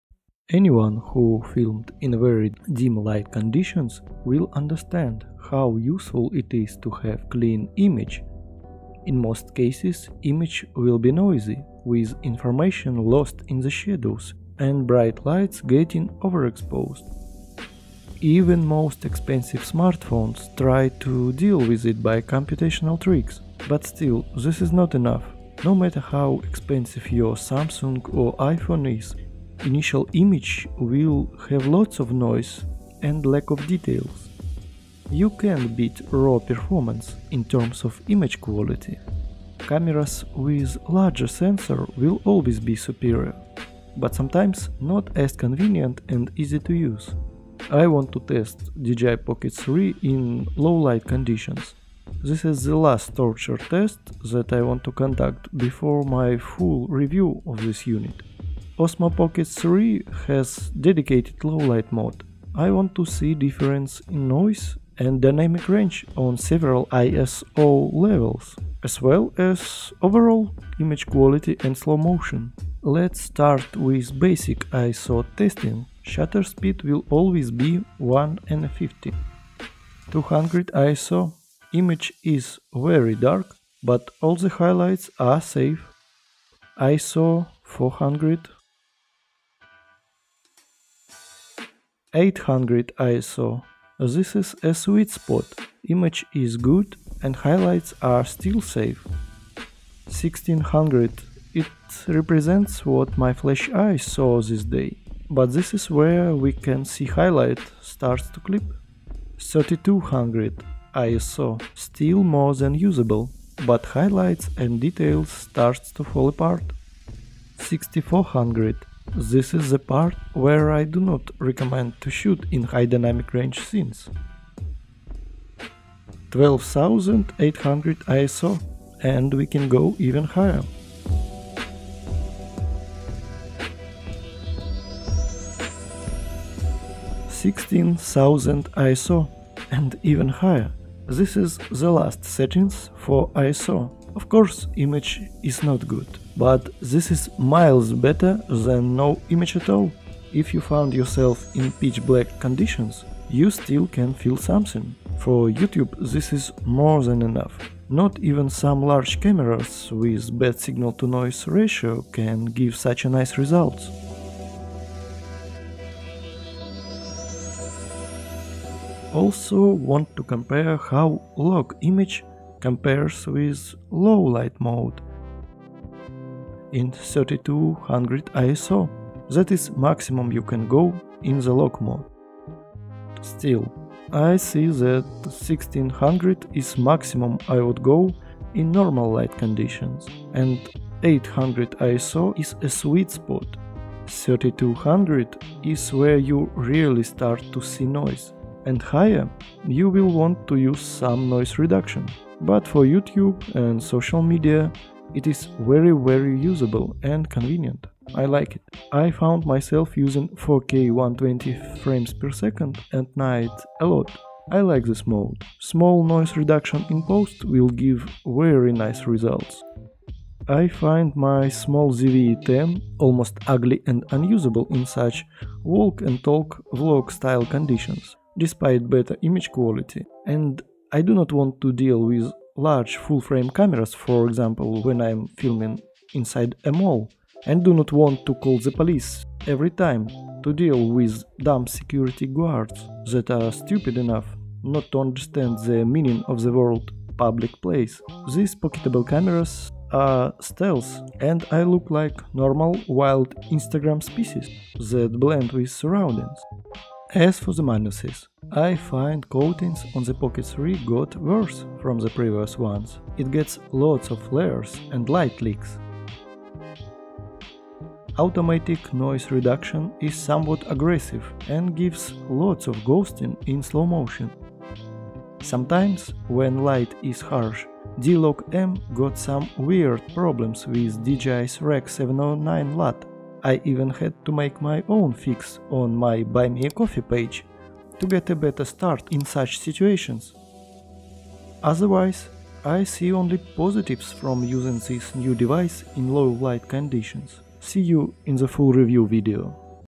Best Camera for low light vlogs 🐙 - Osmo Pocket 3
From the darkness of our existence comes technology that has vision of the night. Can Dji Osmo Pocket 3 light our hearts to see the truth of our world?
Or will we forever be stuck in the noisy overexposed reality of our past?

In this video we will look at the Dji Osmo pocket 3 and compare ISO levels and low light modes

0:00 - Clean image is important
0:17 - Phones and sensor size
0:46 - Test conditions
1:19 - ISO Test 200-3200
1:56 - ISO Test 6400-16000
2:48 - D-log M vs Lowlight Mode
3:28 - Slow motion 4k 120 fps
3:39 - ZVE-10 too shaky
3:49 - Stealth mode
4:16 - Problems with Dji Osmo Pocket 3
4:34 - Fix for the overexposure of D-log M

Alien Technologies